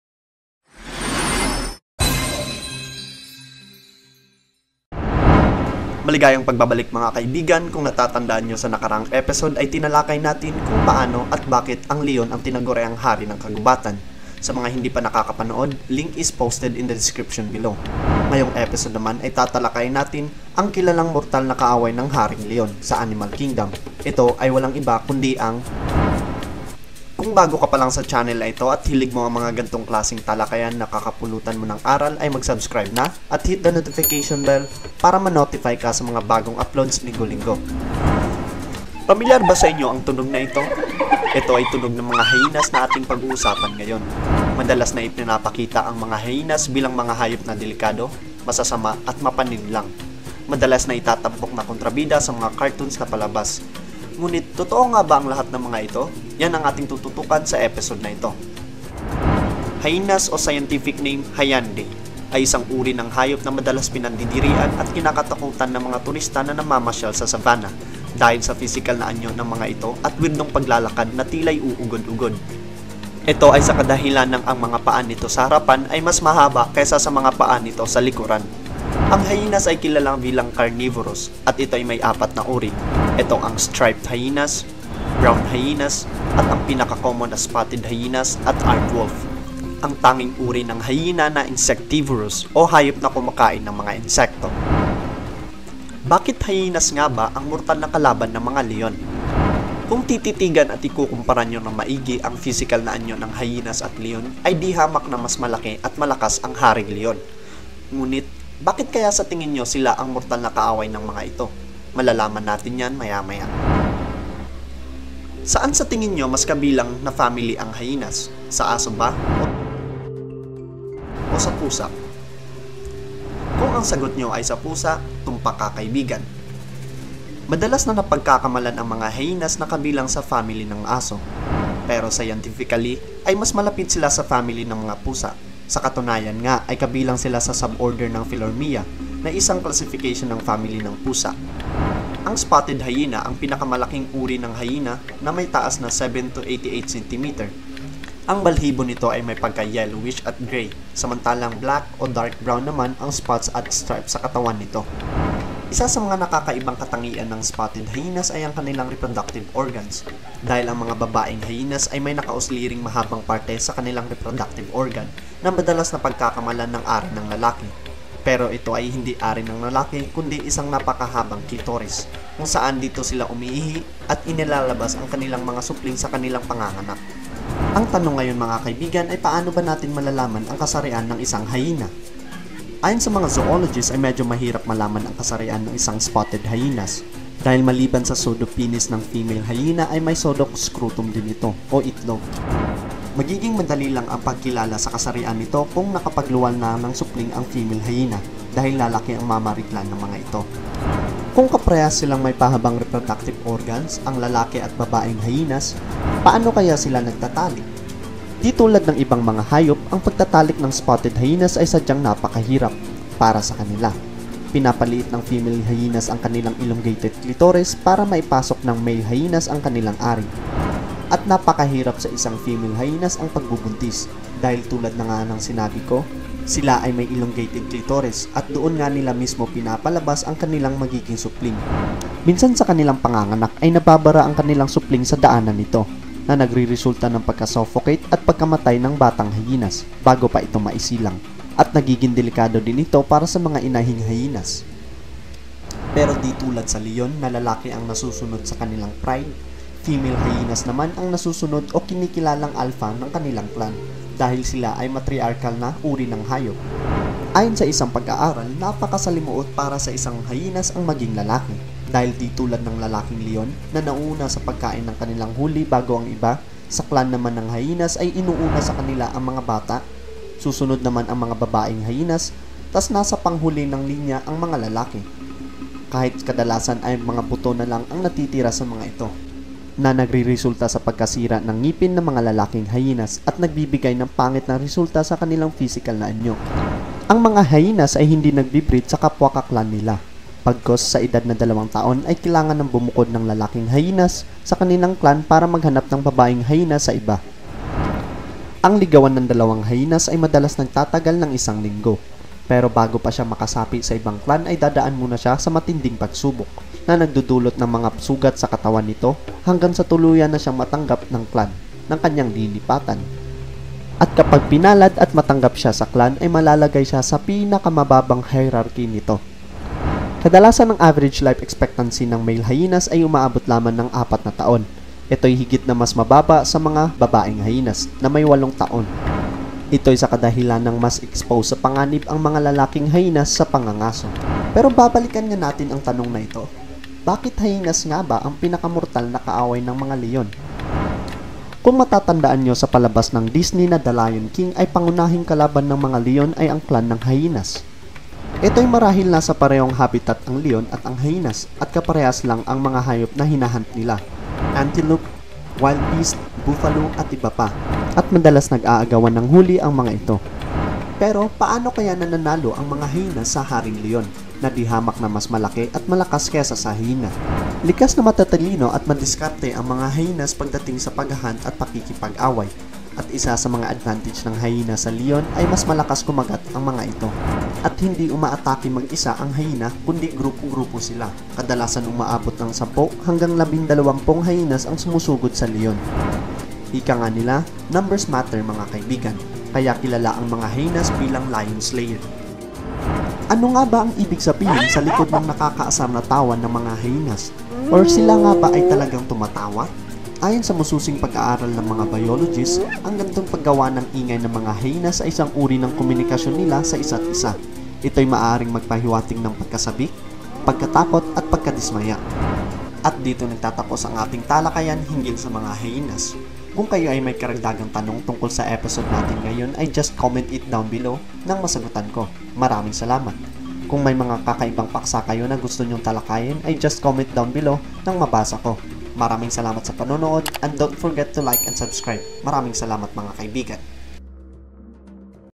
HYENA! ANG HAYOP NA KATAPAT NG MGA LEON | TALAKAYAN PH
Maligayang pagbabalik mga kaibigan, sa episode na ito ay tatalakayin natin ang hayop na mortal na kaaway ng mga LEON! Aalamin natin kung bakit HYENA ang kanilang katapat sa animal kingdom.